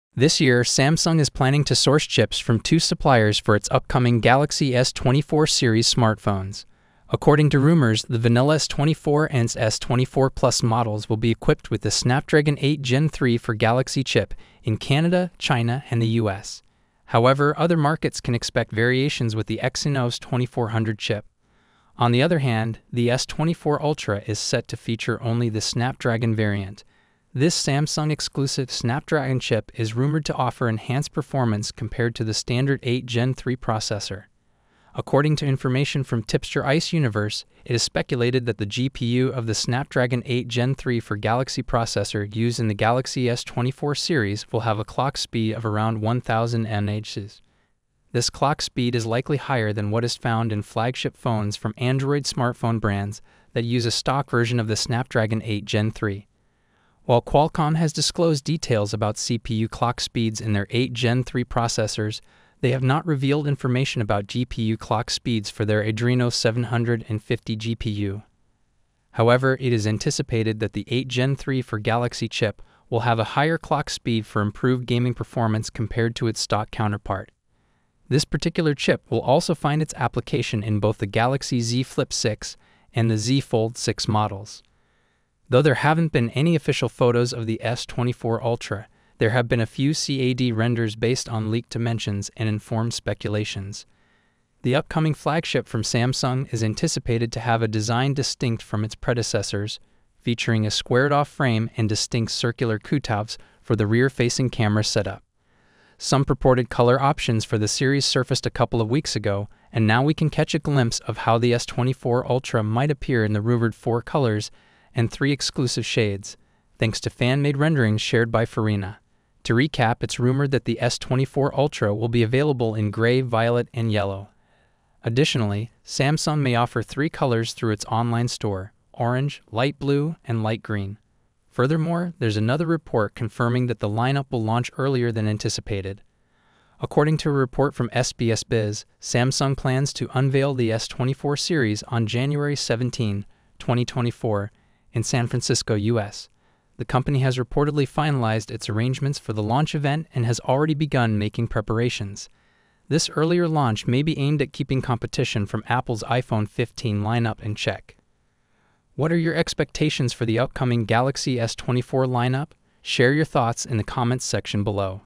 Samsung Galaxy S24 Ultra - YES OFFICIALLY CONFIRM!
Samsung Galaxy S24 Ultra is going to launch globally with Snapdragon 8 Gen 3 chipset, unlike the S24 and S24+, which will have the Exynos 2400 in some regions. The interesting bit is the new Snapdragon 8 Gen 3 normally has its prime core clocked at 3.2 GHz, so the Samsung-exclusive "for Galaxy" version might get an extra 100 Mhz boost. A GPU overclock is also a possibility.